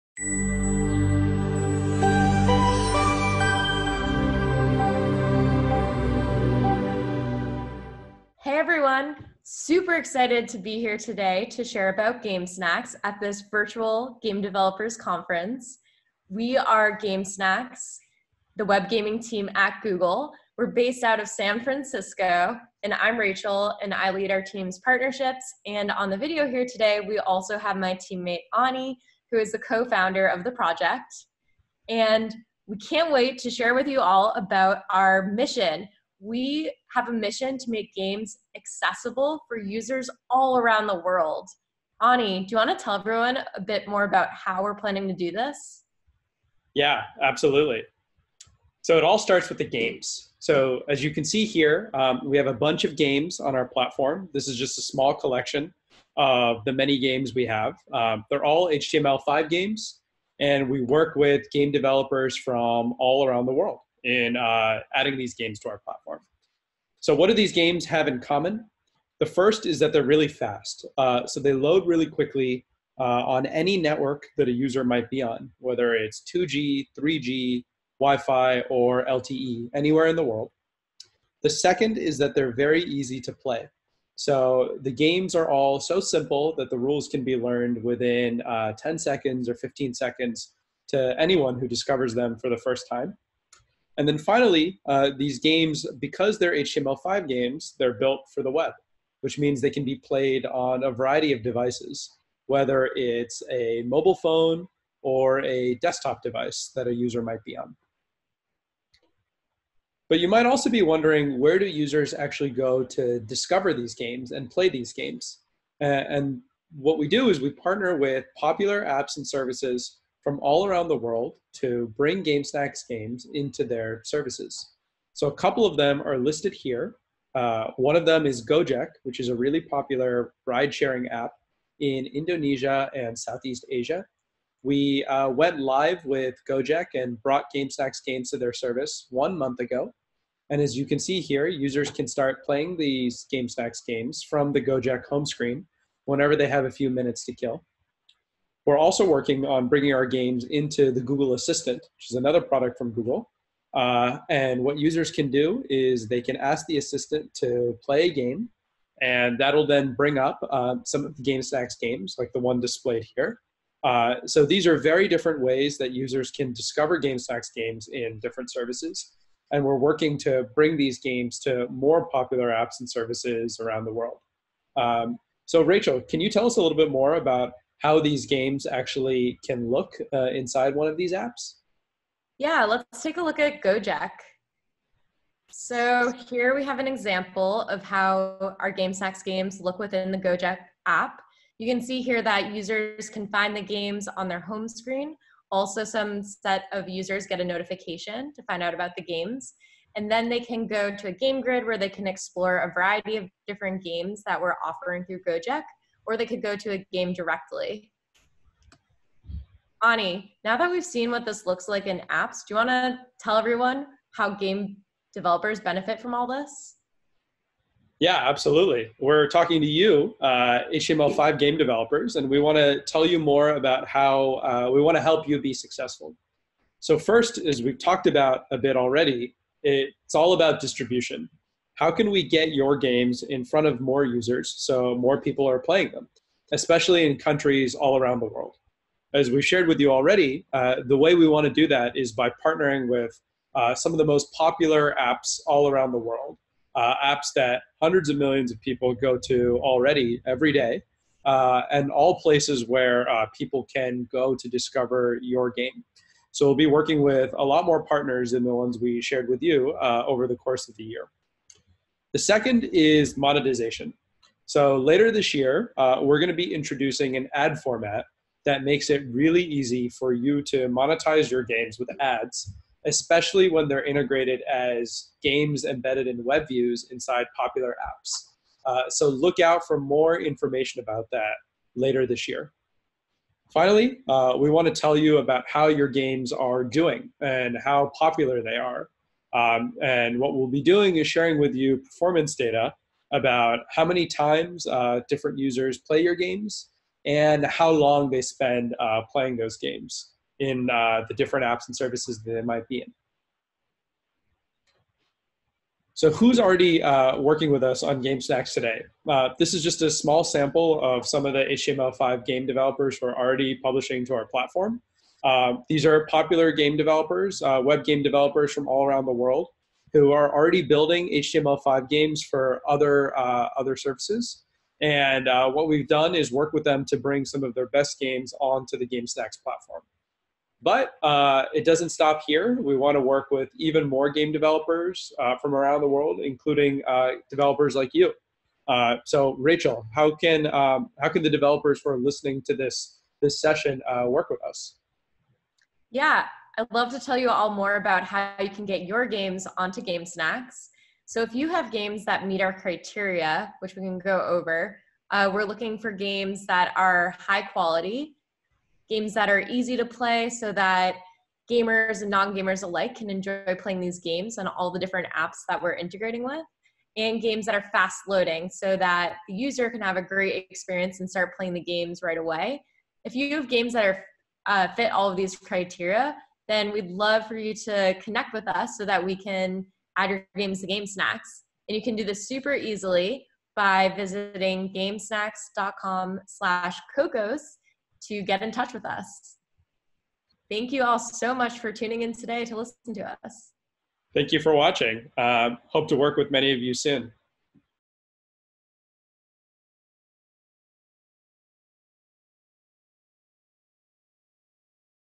Making Games Accessible to Everyone With GameSnacks – Ani Mohan and Rachael Bornstein of Gamesnacks
This talk is part of the Cocos Game Developer Connect online conference

Ani Mohan and Rachael Bornstein sit down and discuss their new project from Google’s Area 120 group, GameSnacks. GameSnacks are HTML5 games that are designed to load quickly from any network and are accessible to anyone.

You can learn more about the service at their website and sign up for their service

About Ani Mohan
Ani Mohan is co-founder and general manager of the GameSnacks team at Google. Before founding GameSnacks, Ani was a product manager at Google in San Francisco, working on Google Chrome and Android. He has also started businesses in personal finance, nutrition, and student tutoring. Ani holds a B.S.E. in Electrical Engineering and a B.S. in Economics from Duke University, which he attended on a full merit scholarship as an A.B. Duke Scholar. Outside of work, he enjoys film, music, writing, and hiking.

About Rachael Bornstein
Rachael Bornstein leads partnerships for the GameSnacks team at Google. In this role, Rachael manages growth of the GameSnacks game catalogue and game integration partnerships with apps. Prior to joining GameSmacks, she co-founded a visual shopping project built out of Google’s Area 120. She’s also worked as an account manager at Google, where she specialized in profitable customer acquisition and had the privilege of working with hundreds of businesses across various industries on their digital marketing strategy. Rachael graduated from Dartmouth College with a B.A. in Biology modified with Mathematics. Outside of work, she is an avid skier, and passionate about entrepreneurship and women in tech.